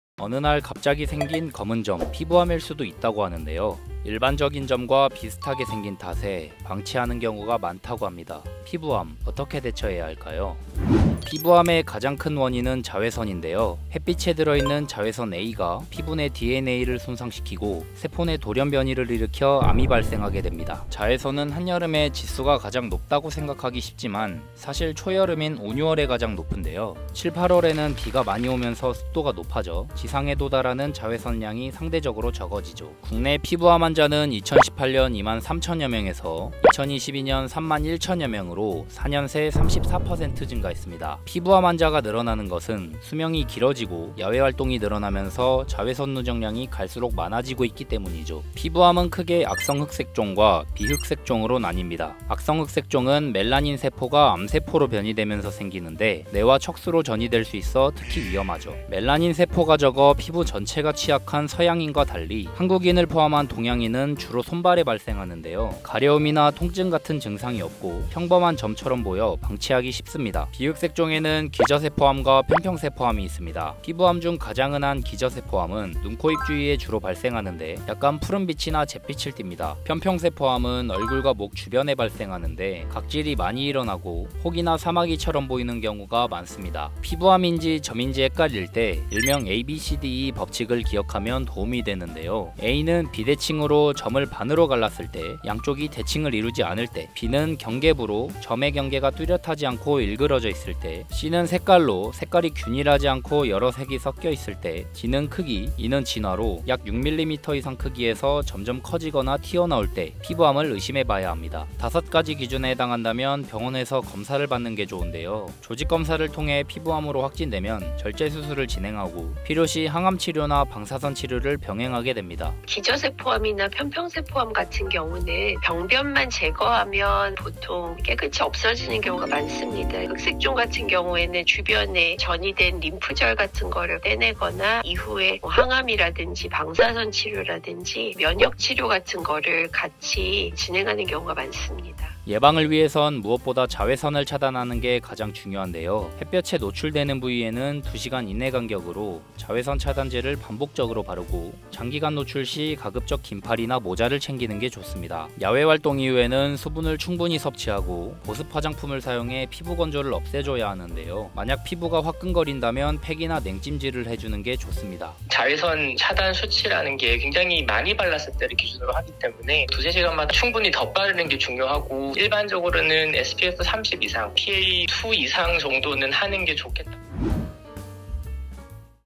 [리빙톡] 점인 줄 알았는데 '피부암'?…초여름에 더 위험
(서울=연합뉴스) 어느 날 갑자기 생긴 검은 점. 피부암일 수도 있다고 하는데요.
일반적인 점과 비슷하게 생긴 탓에 방치하는 경우가 많다고 합니다.
자외선이 가장 큰 원인으로 꼽히는 피부암, 어떻게 대처해야 할까요?

류재갑 기자 서자빈 인턴기자 오유빈 크리에이터

00:00 인트로
00:11 피부암의 원인
00:33 국내 피부암 환자 추이
00:48 피부암의 종류 및 특징
01:27 피부암 자가 진단 방법
01:52 피부암 치료 방법
02:02 전문가 인터뷰(1)
02:22 피부암 예방법
02:45 전문가 인터뷰(2)